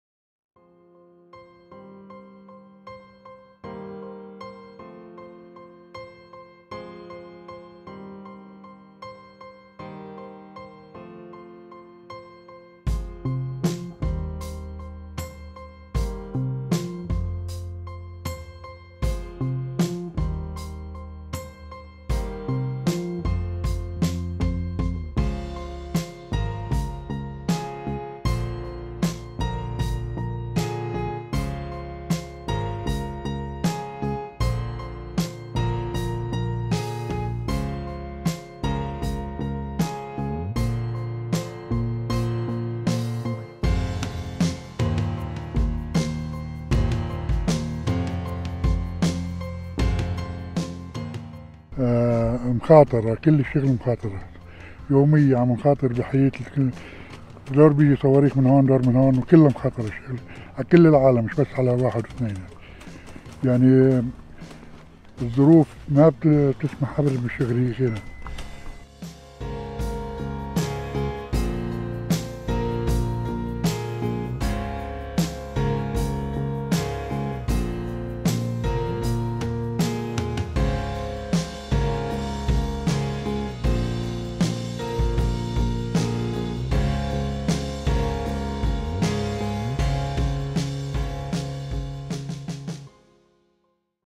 Lebanese farmers face olive season loss due to ongoing Hezbollah-Israeli conflict
Lebanese Agriculture Minister Abbas Hajj Hassan said that the ongoing Israeli airstrikes have devastated olive farmers, who have lost two consecutive seasons.